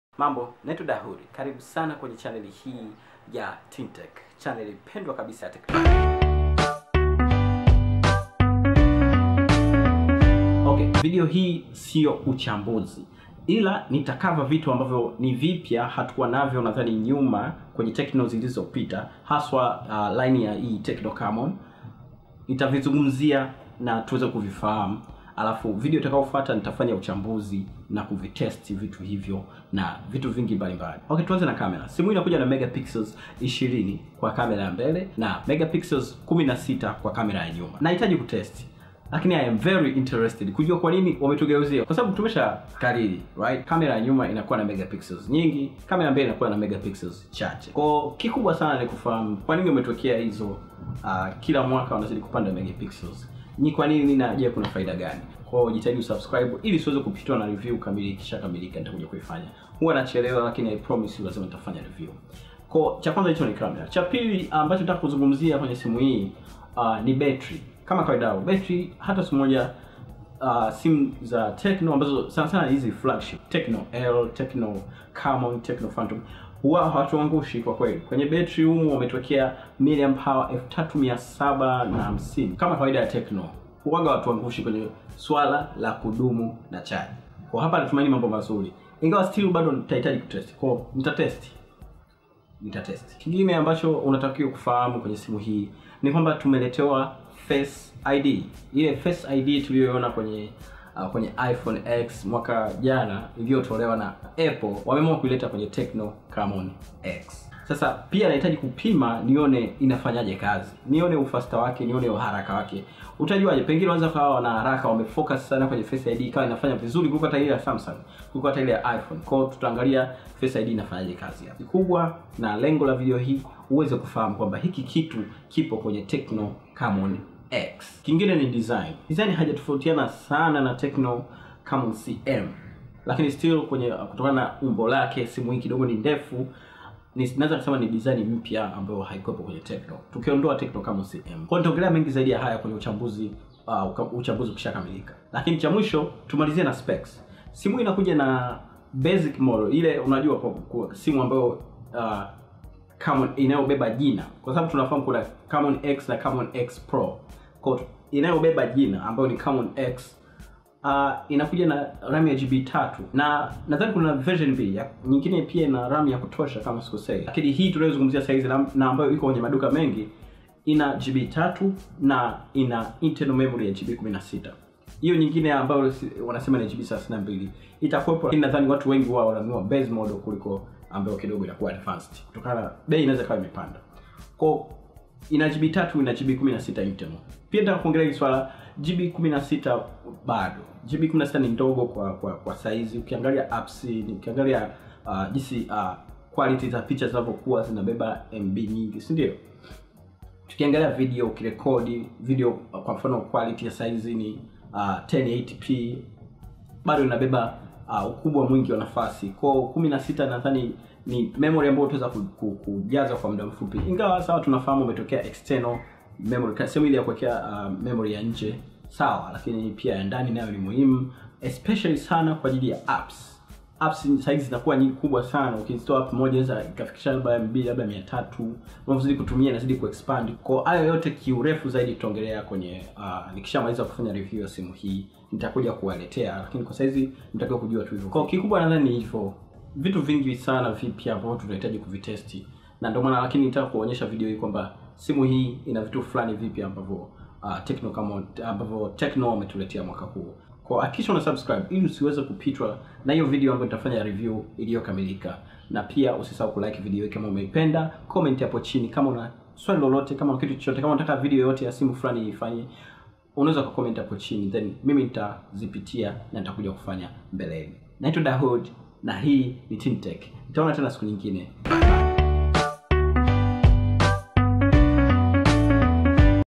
TECNO CAMON X - Kipi Kipya Mwaka Huu.?
Asante kwa kuangalia video hii unaweza kufuata Teen Tek katika ukurasa wa Twitter @TeenTekhq

TECNO CAMON CM KIUNDANI ZAID

Fahamu Vitu VIpya Katika Simu Ya TECNO CAMON X"